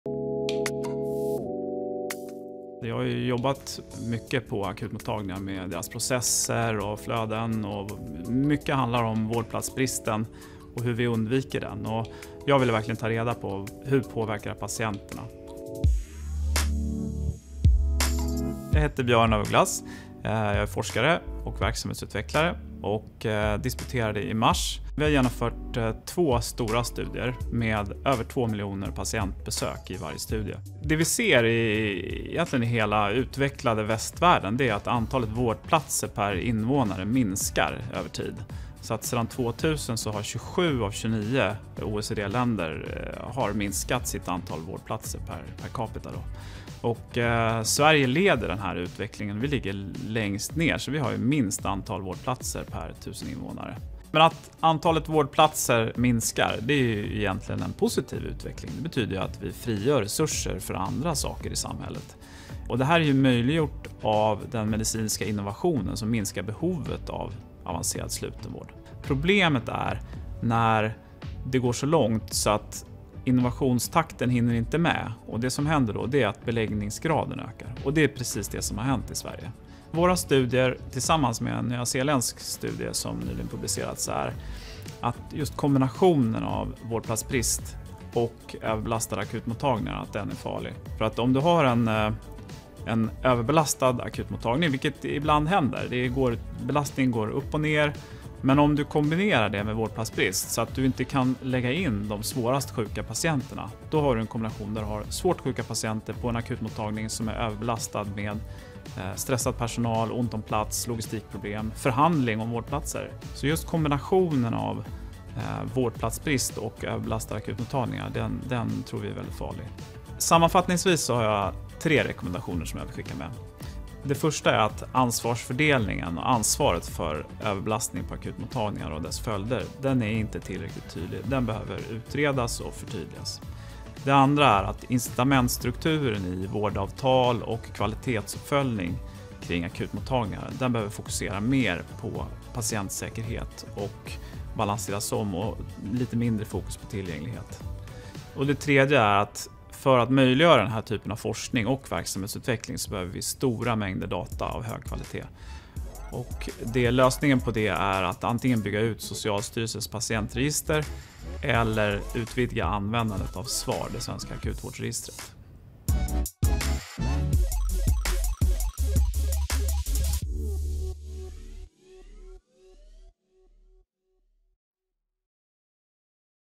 Forskaren förklarar: Björn af Ugglas om vårdplatsbrist, överbelastning och patientsäkerhet
I en ny SNS-rapport undersöker forskaren Björn af Ugglas sambandet mellan vårdplatsbrist, överbelastade akutmottagningar och patientsäkerhet. Vilka blir konsekvenserna när antalet vårdplatser minskar snabbare än behovet av slutenvård? Och vem bär ansvaret för kapacitetsbrist och överbelastning på akutmottagningarna?

SNS-rapporten ”Hur påverkas patientsäkerheten av överbelastade akutmottagningar och vårdplatsbrist?” går att ladda ner här